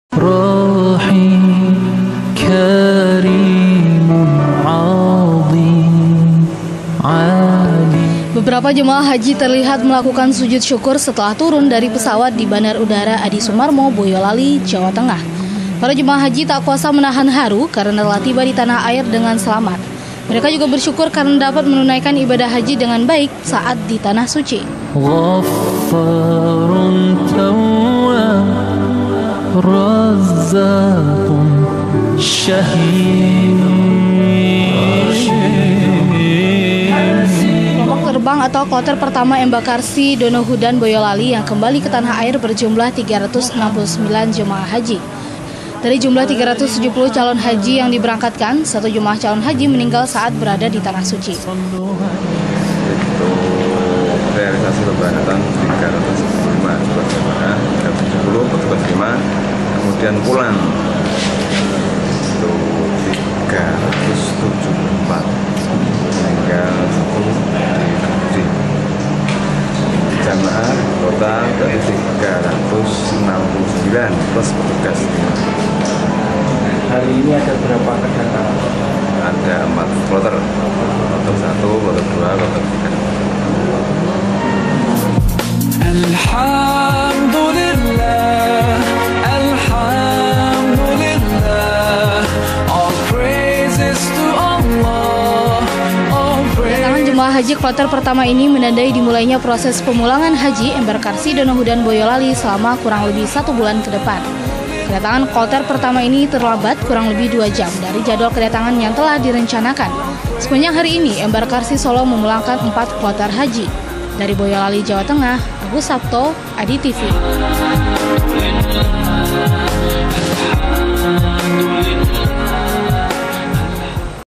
KEPULANGAN HAJI
Beberapa jamaah haji terlihat melakukan bersujut shukur setelah turun dari pesawat di bandara Adisumarmo boyolali jawa tengah, para jamaah haji tak kuasa menahan haru telah tiba di tanah air dengan selamat, mereka juga bersyukur karena mereka bisa melaksanakan ibadah haji dengan baik saat disaat di tanah suci.